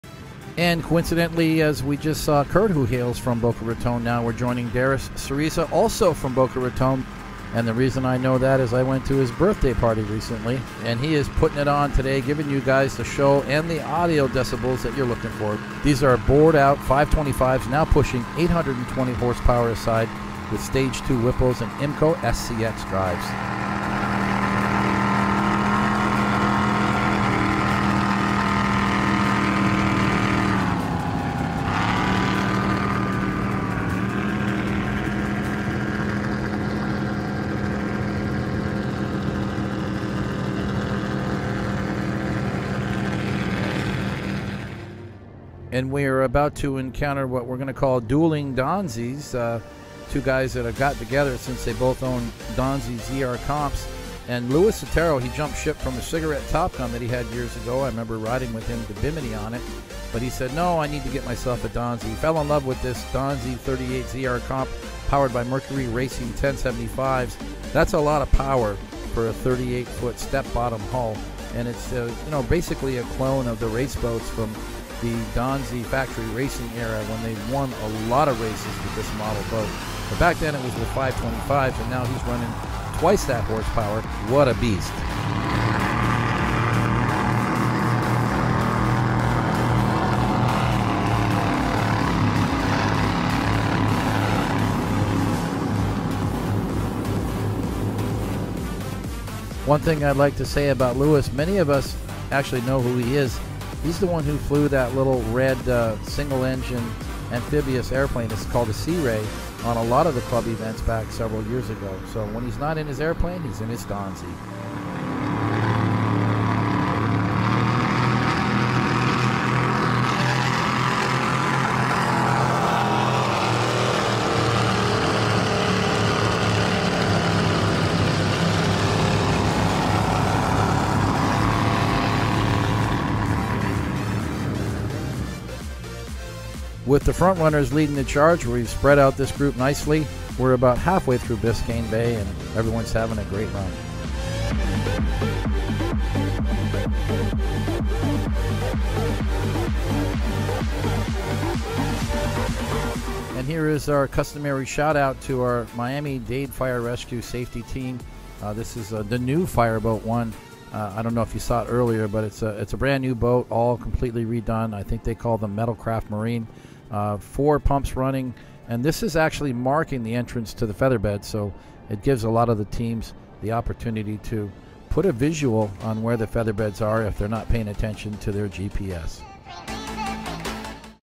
The two fastest Donzi’s in South Florida battling head to head!
Here is a cool clip from last November key west poker run.
The black Donzi is powered by twin 1075s
The Blue Donzi is powered by twin  825s

Thank you Stu for another amazing poker run 🤙🏽🚤💨